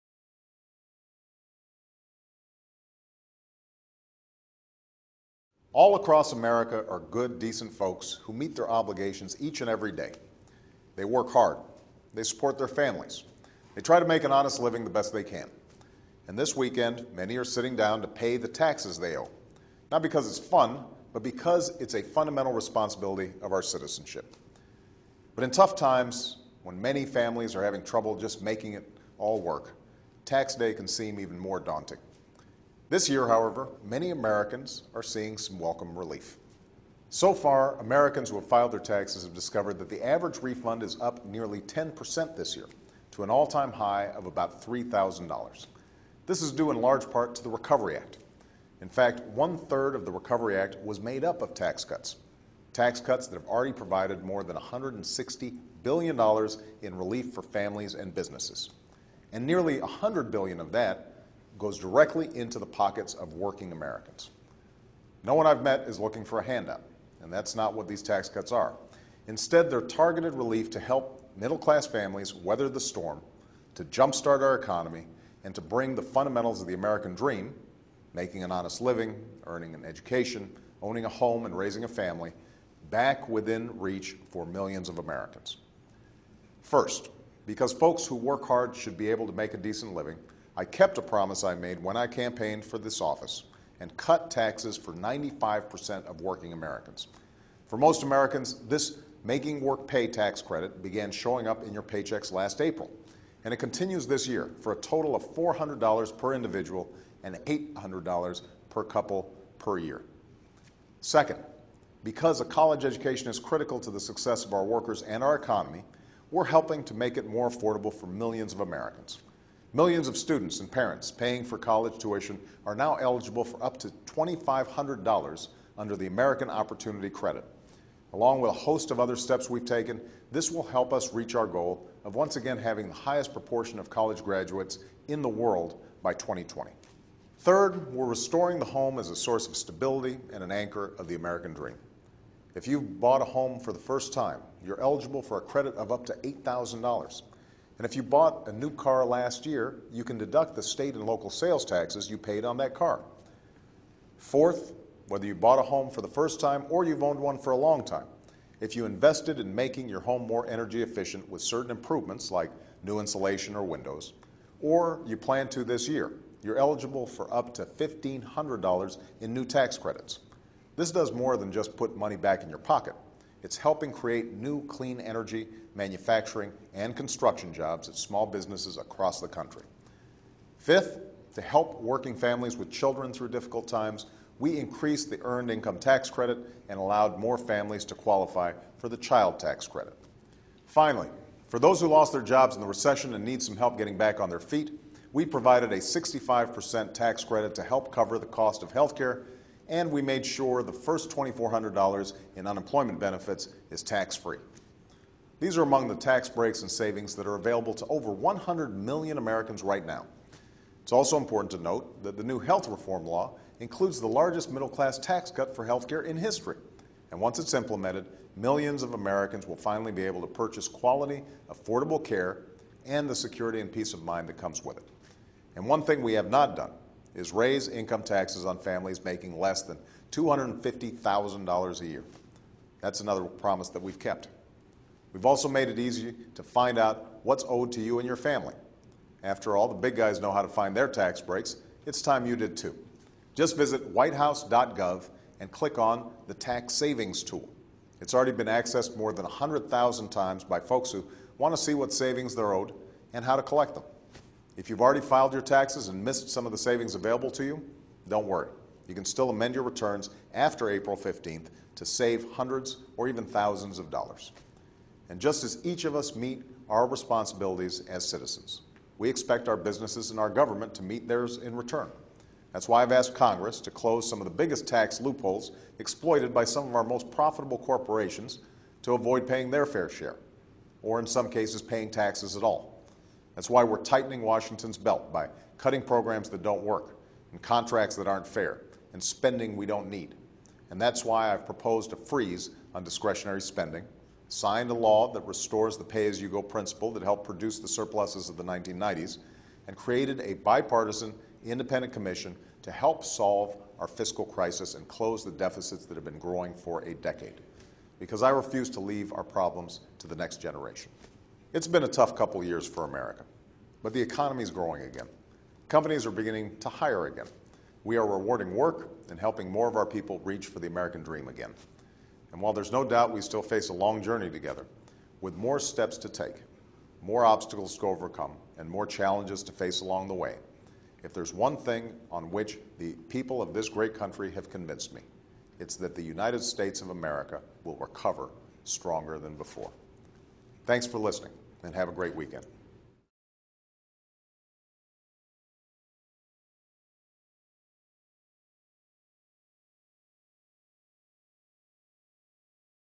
Pr. Obama - Apr 15, 100 mil. citizens to get Tax Cuts from Recovery Act
As the deadline for filing federal income tax returns approaches, President Obama used his weekly online address to talk about how Americans can benefit from the tax credits offered in the economic stimulus package.
Washington, DC : 5 min.